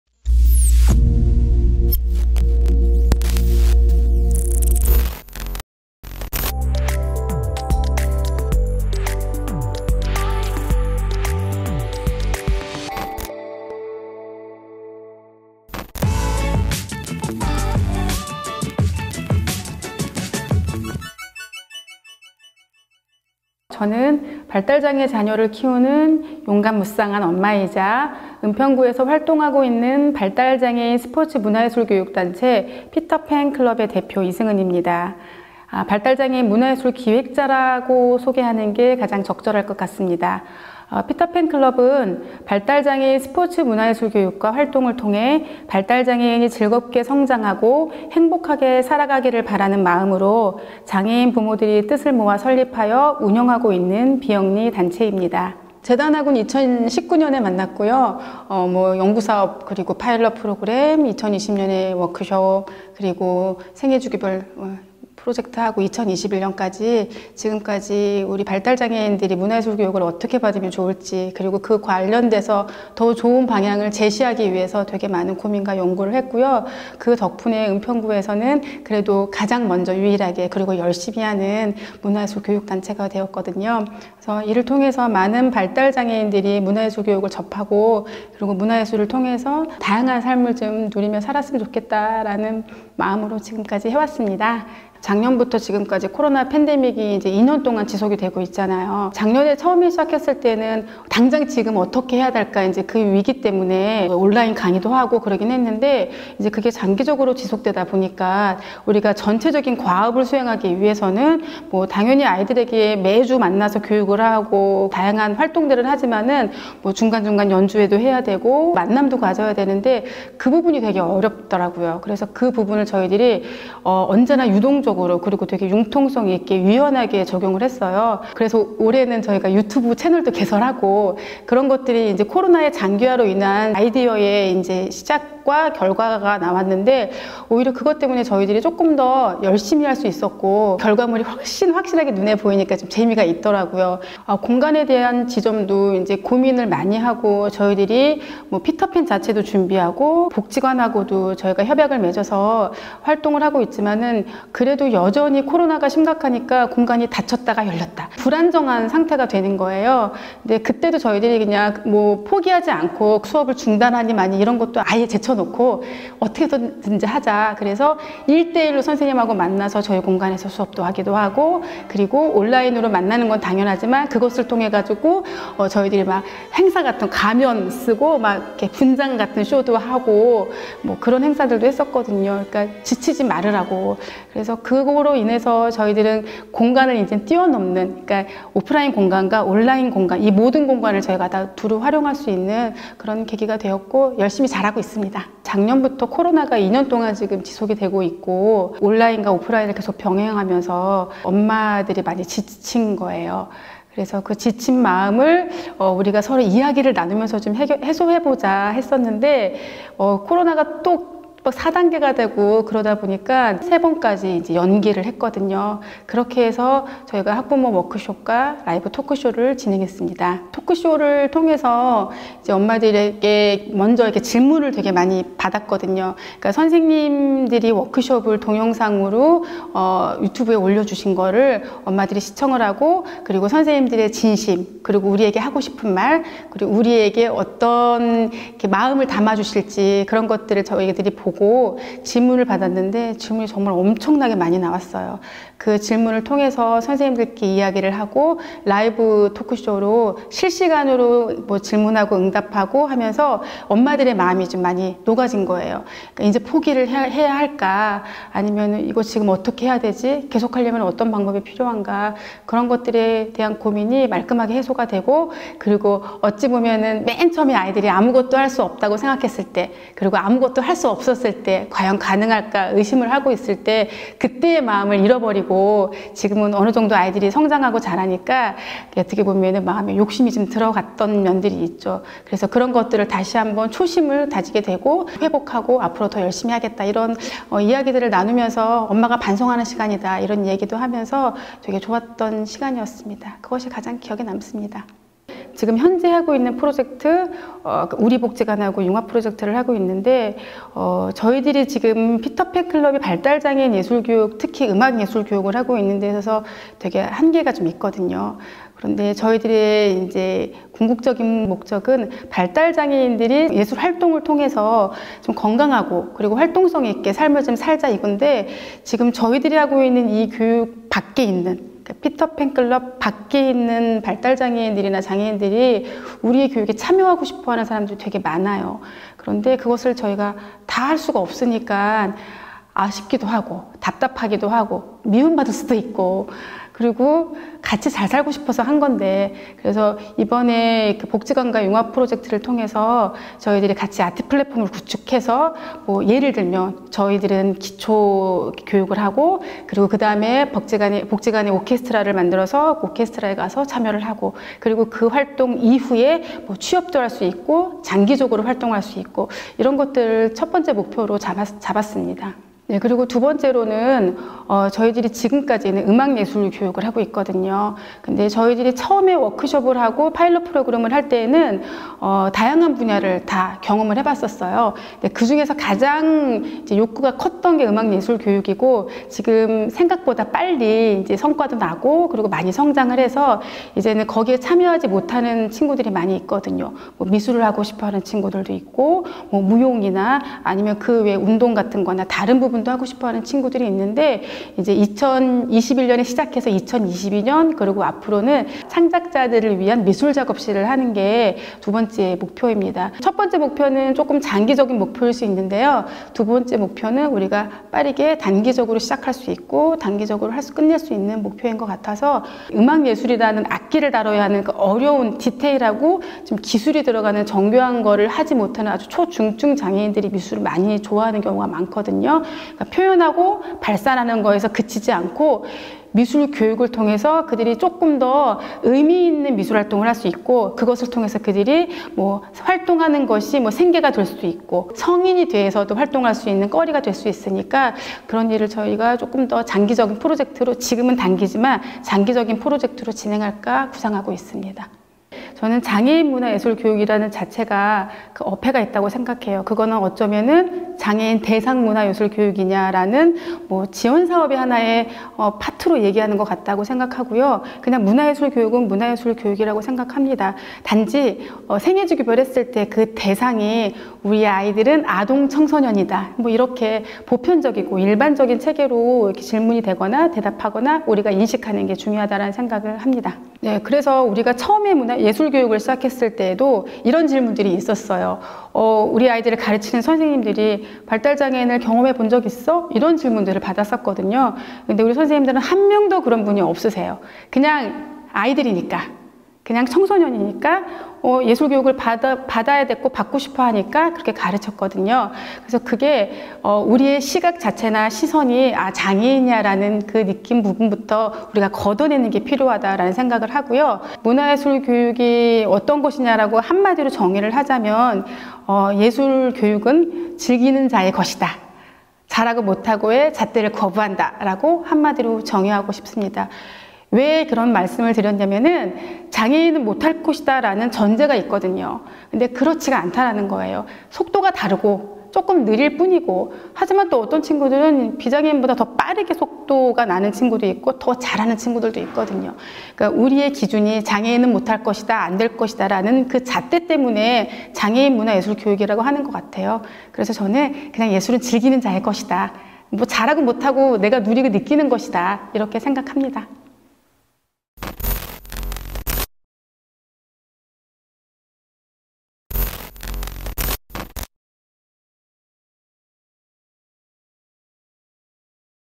[은평 지역문화예술 위크 WEEK] 피터팬클럽 (장애아동청소년 음악예술교육 지역자원 융합 프로젝트)_세션1. 은평 지역문화예술 콘텐츠 공유회_생애주기별 문화예술교육 프로젝트
[은평 지역문화예술 위크 WEEK] 세션1.은평 지역문화예술 콘텐츠 공유회

생애주기별 문화예술교육 프로젝트

피터팬클럽 _장애아동청소년 음악예술교육 지역자원 융합 프로젝트_

00:22 단체소개
00:56 은평문화재단과 인연?
01:35 기존 프로젝트와 달랐던 지점
03:17 프로젝트를 진행하며 가장 기억에 남았던 것은?
04:55 앞으로의 계획
07:25 장애인 문화예술교육에 대한 생각

은평 지역문화예술 위크 WEEK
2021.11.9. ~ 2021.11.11
은평문화재단 유튜브, 인스타그램 온라인 개최

▼‘은평 지역문화예술 위크 WEEK’에 대해 더 알고 싶다면?▼